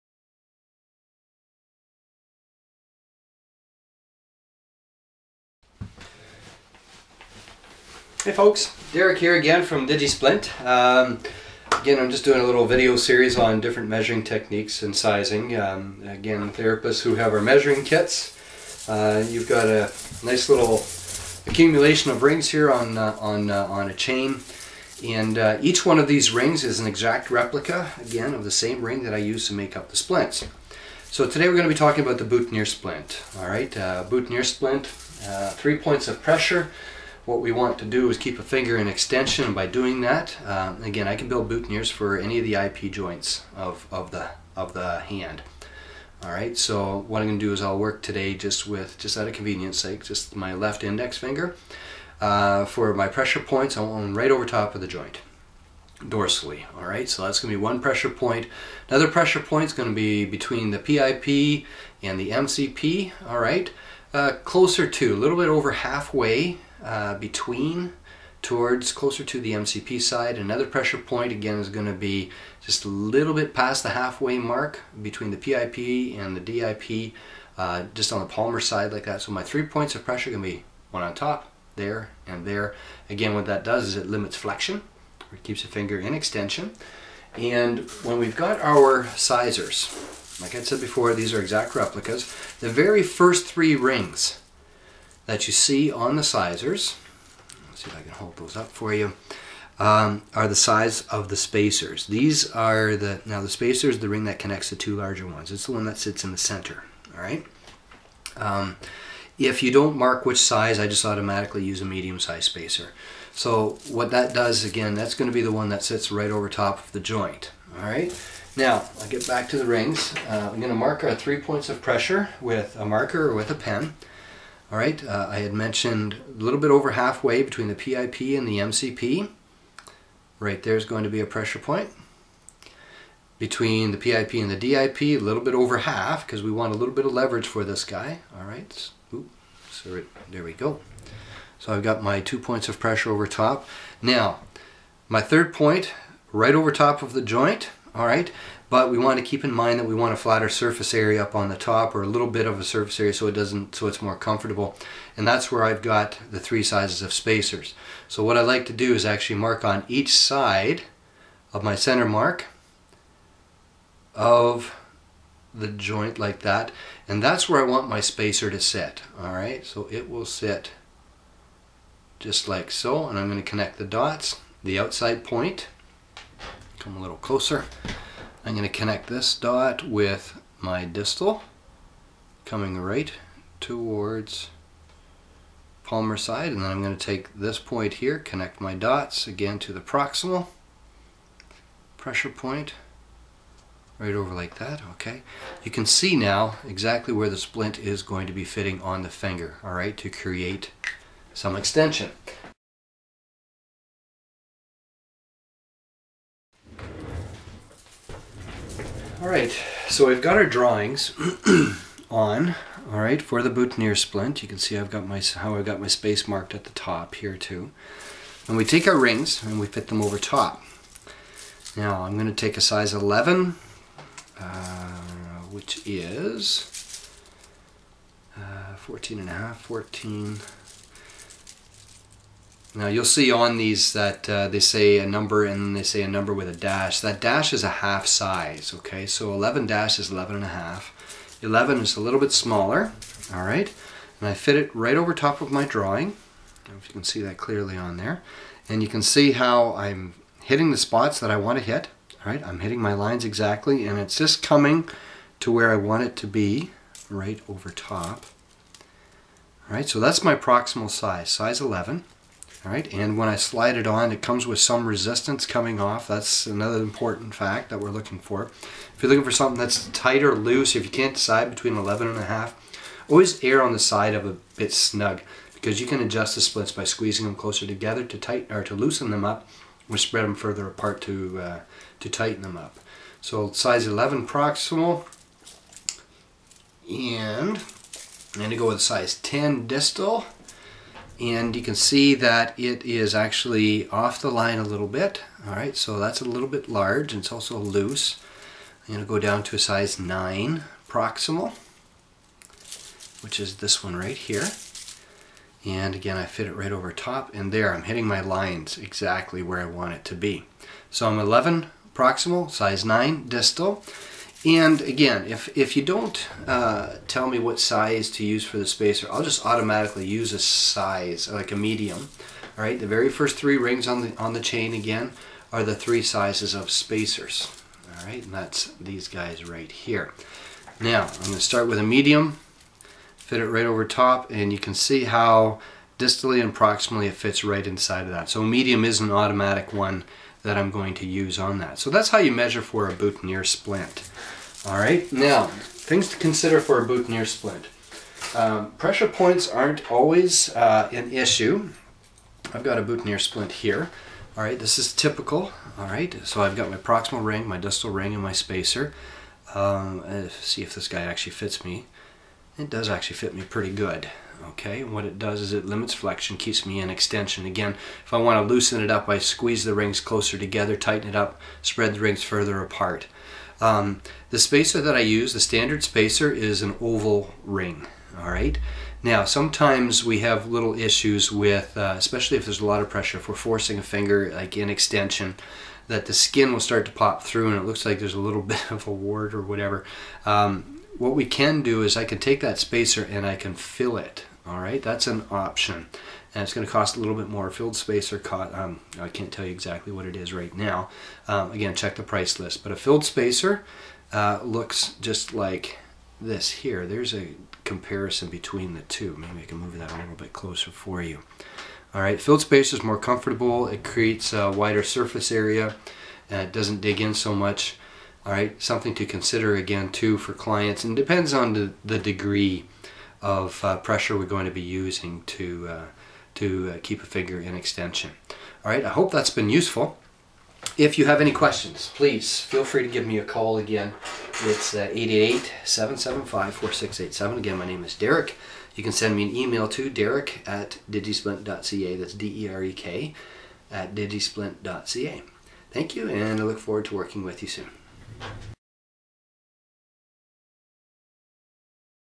Measuring for the Boutonniere Splint
How to measure a finger for a Digisplint Boutonniere Splint